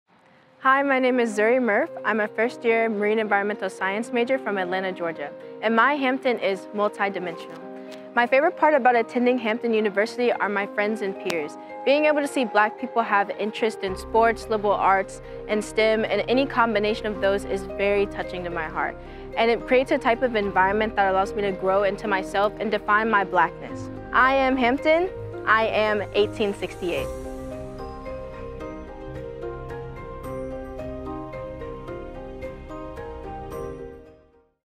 Day of Giving - Tuesday - Students
Listen to why this Marine & Environmental Sciences student's Hampton is multi-dimensional.